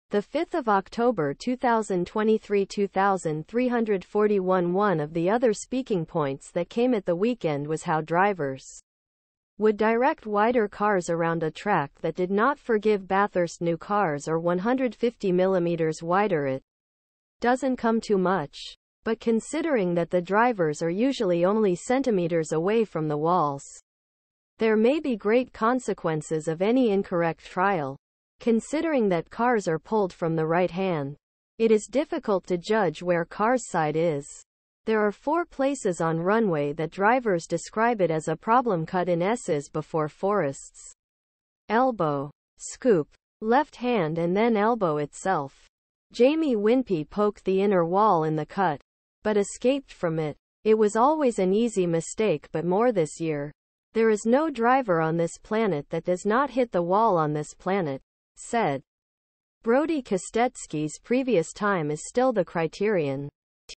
Bathurst 1000 LIVE results 2023: V8 Supercars Practice 3, highlights, latest news; 'Big dramas' as a
05 October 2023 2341  One of the other speaking points that came at the weekend was how drivers would direct wider cars around a track that did not forgive Bathurst. New cars are 150 mm wider - it doesn't come too much, but considering that the drivers are usually only centimeters away from the walls, there may be great consequences of any incorrect trial. Considering that cars are pulled from the right hand, it is difficult to judge where car's side is. There are four places on runway that drivers describe it as a problem - cut in Esses before Forrest's elbow, scoop, left hand and then elbow itself. Jamie Whinp poked the inner wall in the cut, but escaped from it. It was always an easy mistake - but more this year. "There is no driver on this planet that does not hit the Wall on this planet."Said. Brodie Kostecki's previous time is still the criterion.